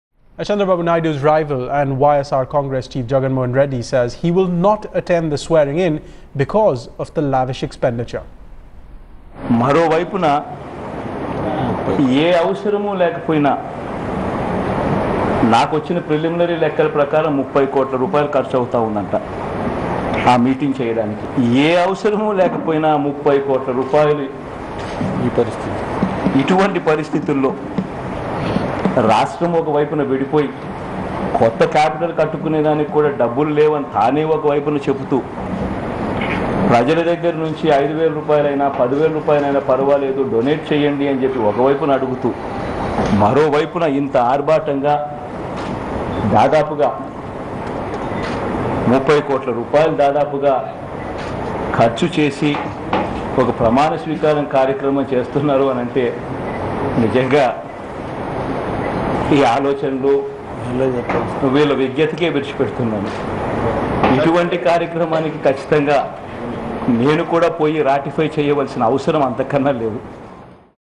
Jagan Mohan Reddy denies to attend Naidu's swearing-in
YSR Congress Chief Jagan Mohan Reddy who is also the rival of Chandrababu Naidu denies to attend the swearing-in ceremony of Naidu stating lavish expenditure the reason behind.